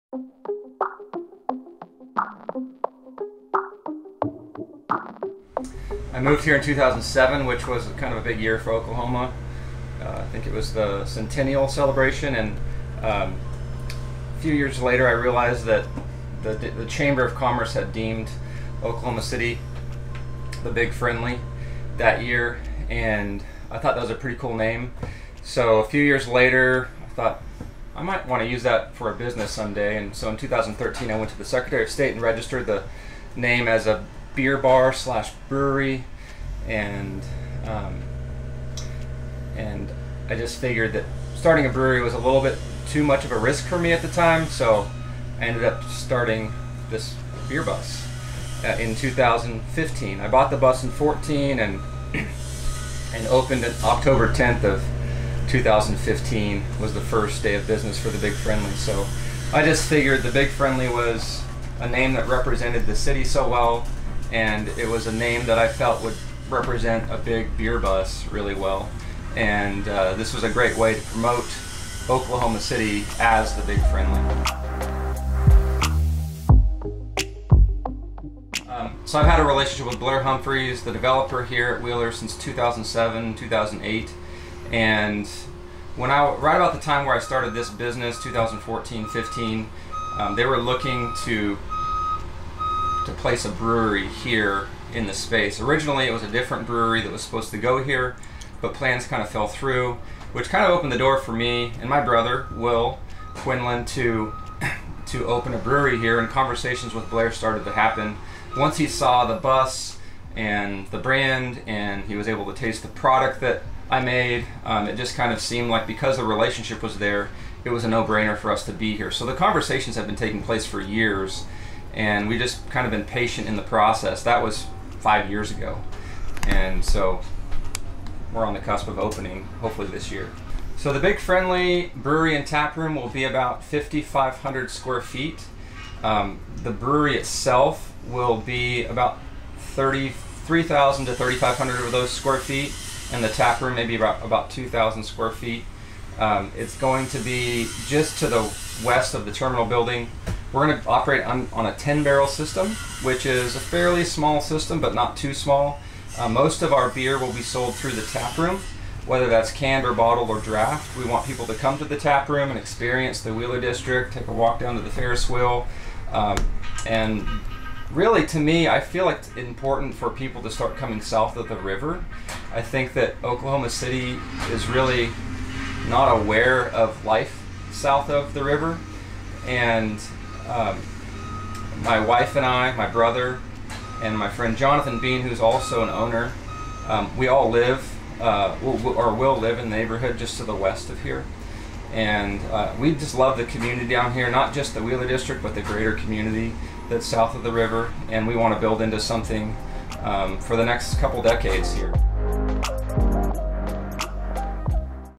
Big Friendly set to open in Wheeler District
Joe Quinlin, owner of The Big Friendly, gives the history of the beer bus and the goal of opening a brewery in the Wheeler District.